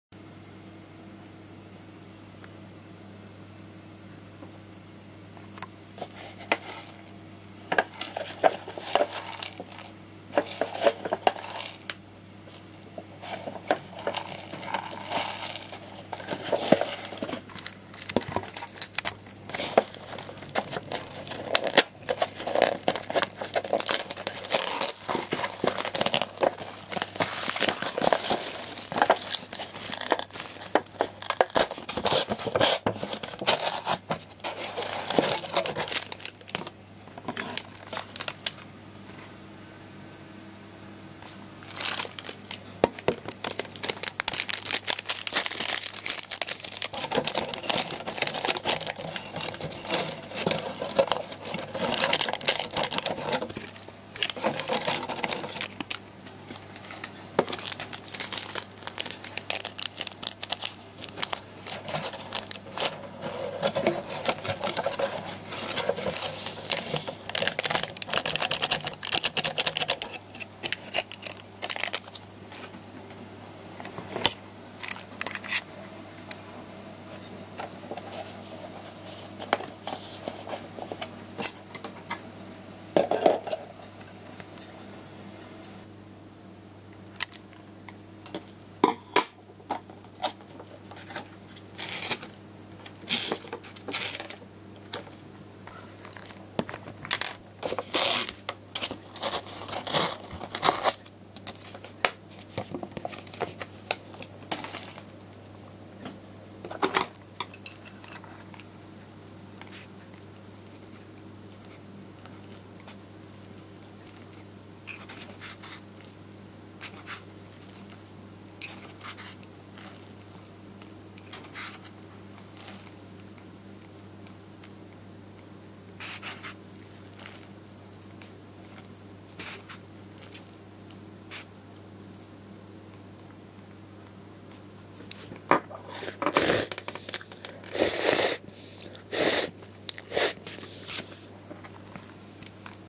【フルHD】サンヨー食品 サッポロ一番 しょうゆ味 2
サッポロ一番しょうゆ味の開封動画です。
こちらはカップめんのほうになります。